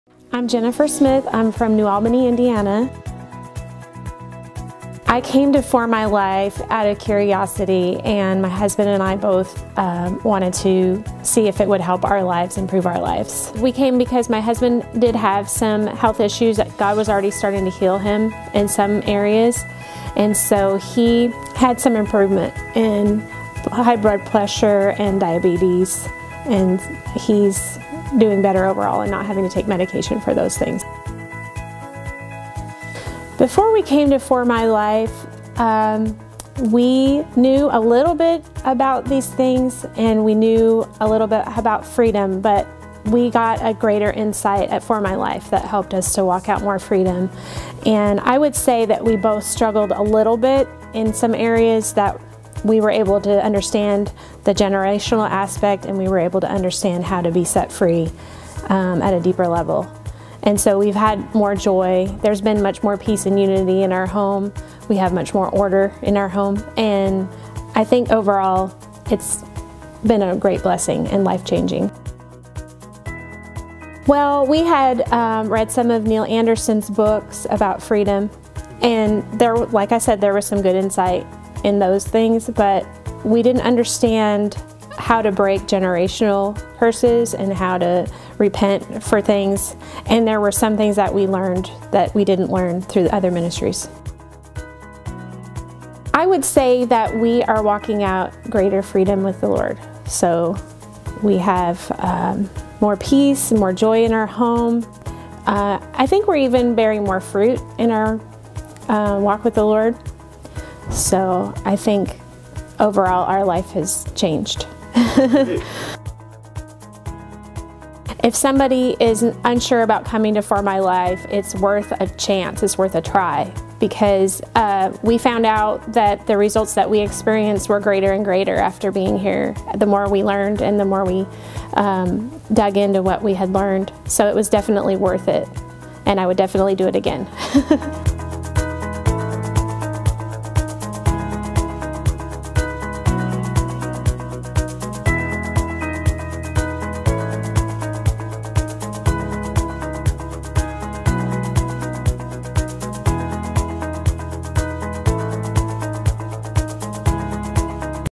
A Life Transformed - Testimony Tuesday
Another amazing testimony from our MI conference that tells the testimony of how curiosity can lead to healing and a deeper connection with the Father.